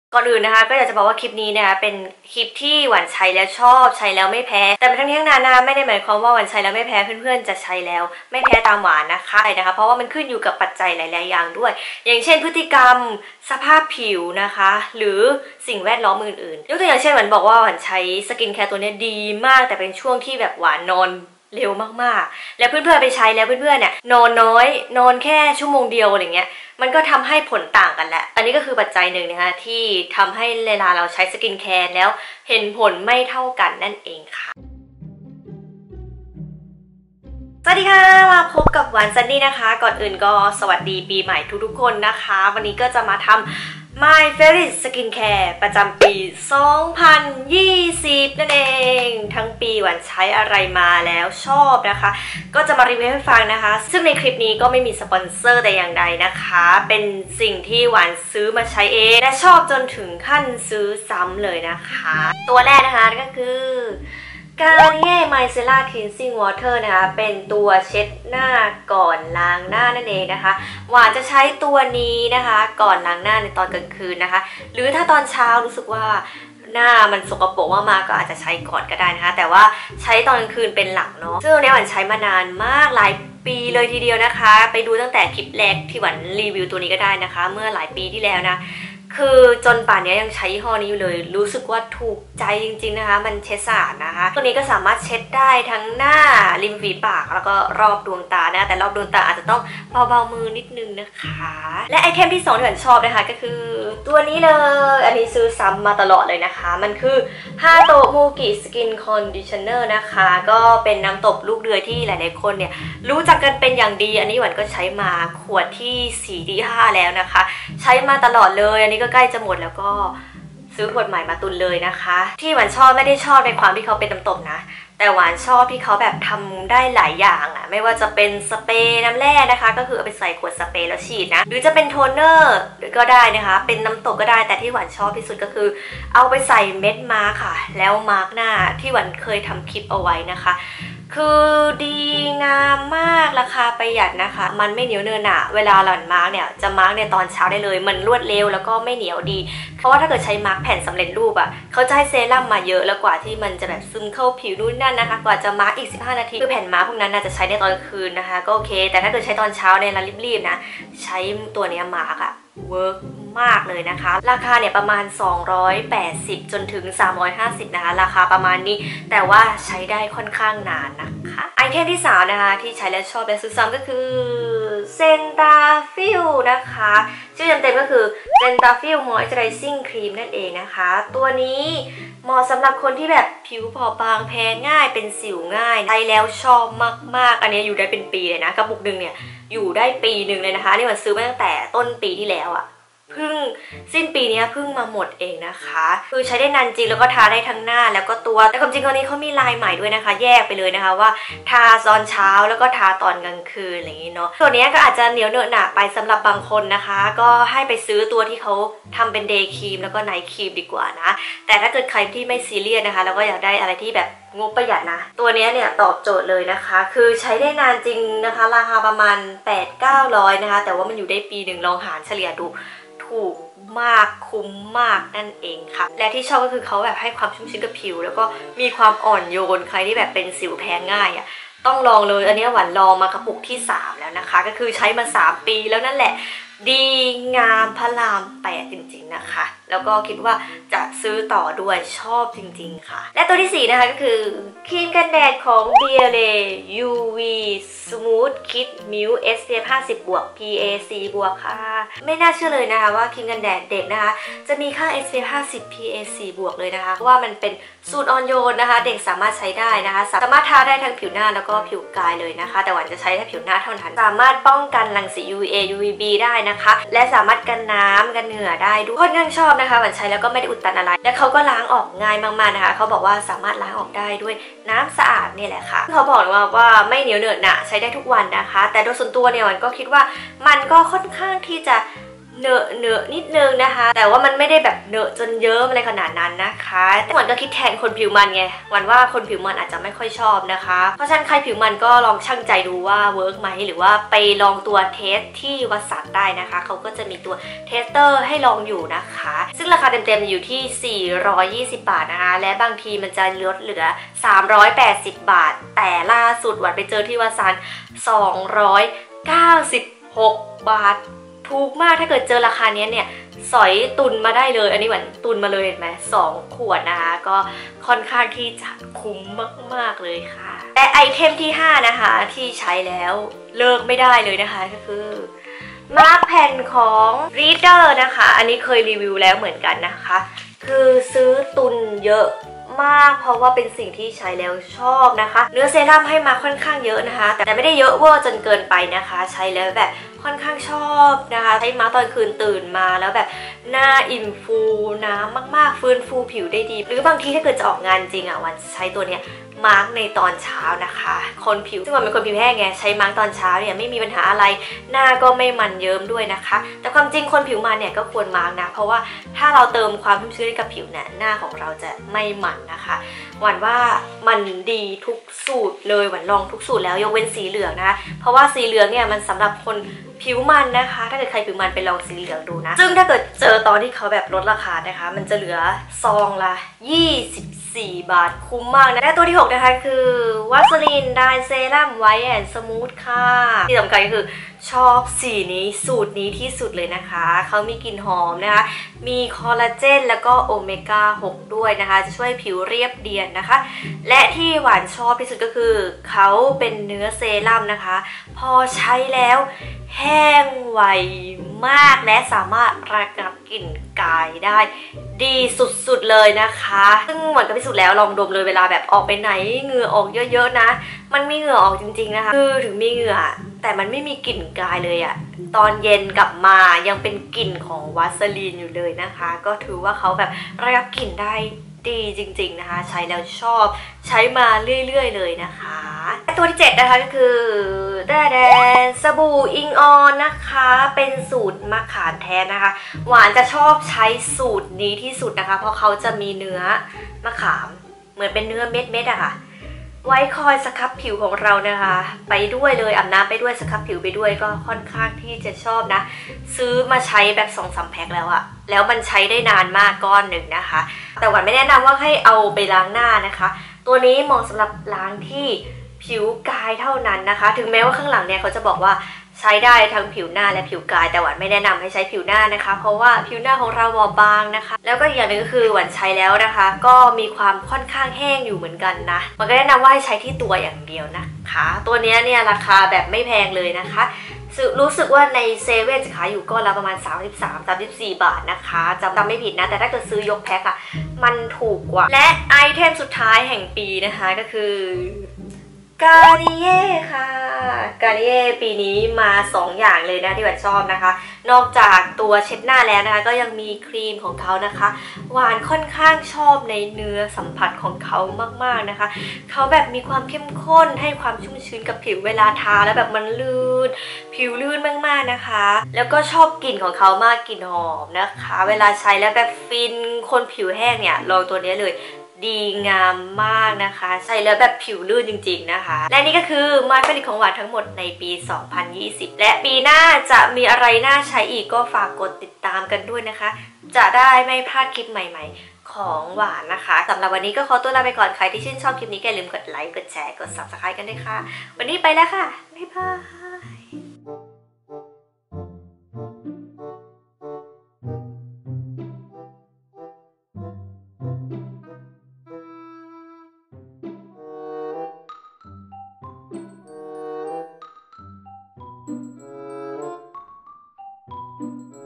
My Favorite Skincare 2020 สกินแคร์ที่ใช้แล้วชอบ ใช้แล้วซื้อซ้ำไม่เคยขาด | WaanSannie
สวัสดีปีใหม่ค่ะ มาช้าแต่มานะ สำหรับคลิป My Favorite Skincare 2020
ทั้งหมดนี้เป็นสกินแคร์ที่ใช้แล้วชอบ ใช้แล้วซื้อซ้ำไม่เคยขาด
ชอบถึงขั้นซื้อตุนไว้เลยจ้า

การ์นิเย่ ไมเซล่า คลีนซิ่ง วอเตอร์ 199 บาท

หวานมีร้านขายที่กรองน้ำฝักบัว ใครที่มีปัญหาเรื่องสิว แพ้น้ำ ไปดูรายละเอียดกันได้นะคะ